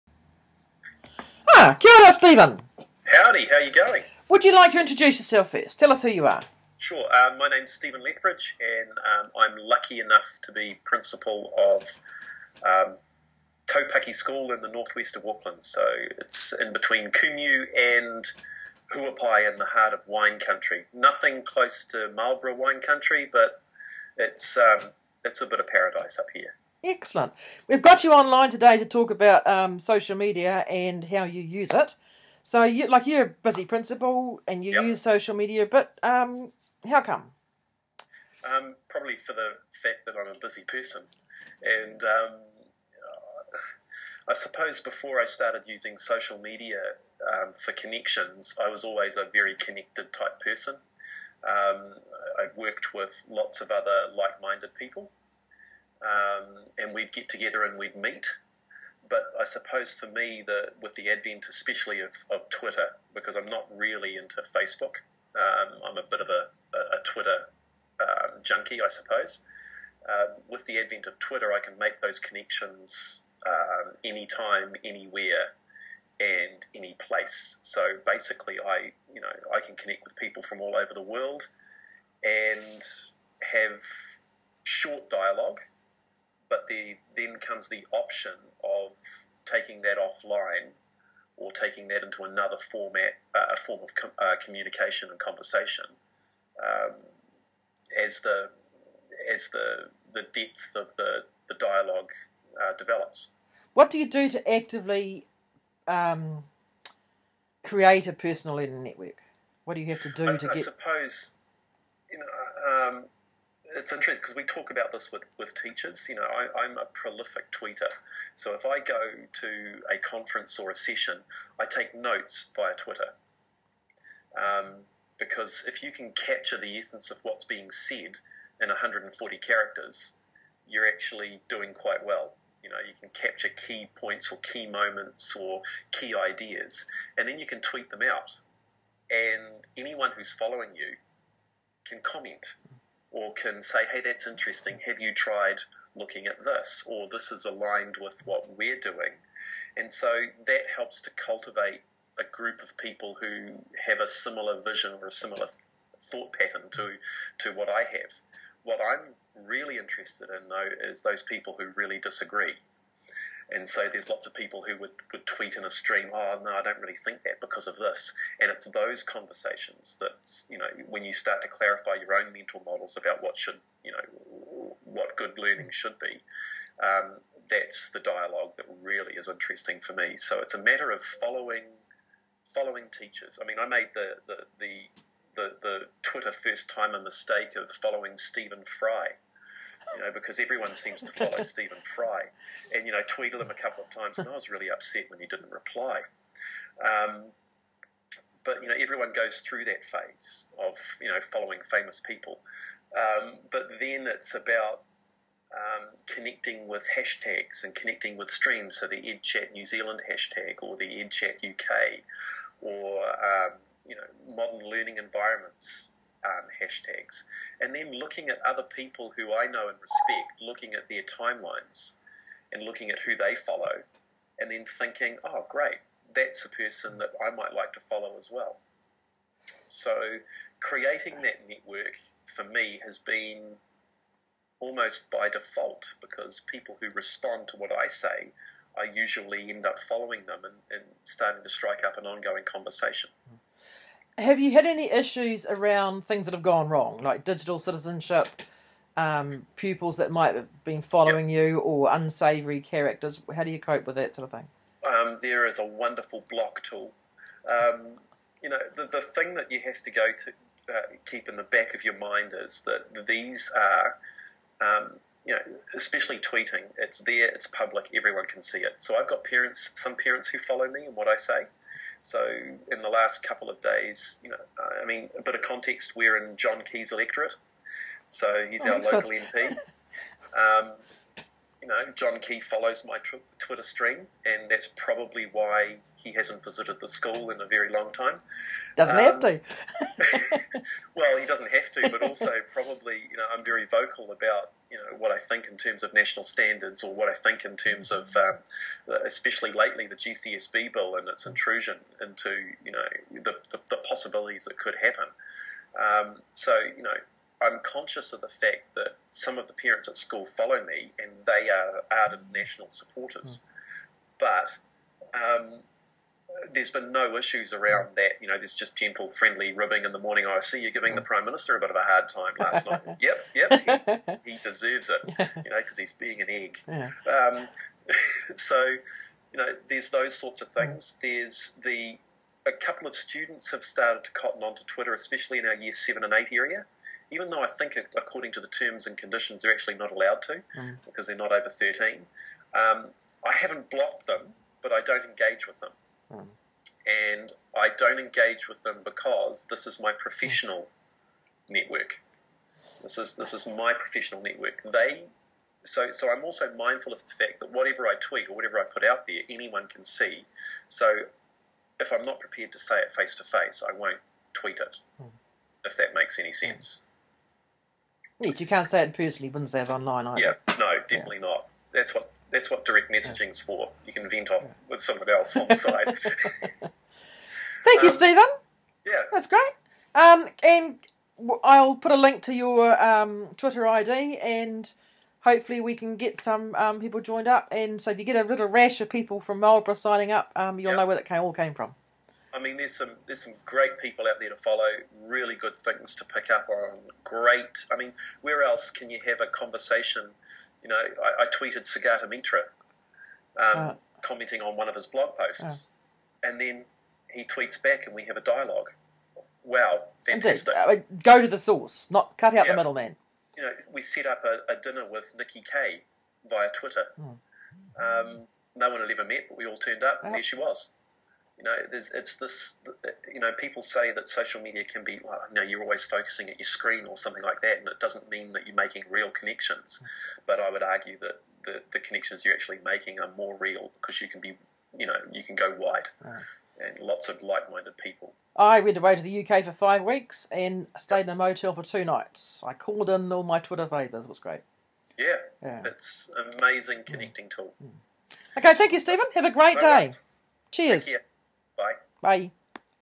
Stephen Lethbridge on Twitter
This video was made to support Marlborough principals and leaders in their exploration of social media to support them as leaders in education.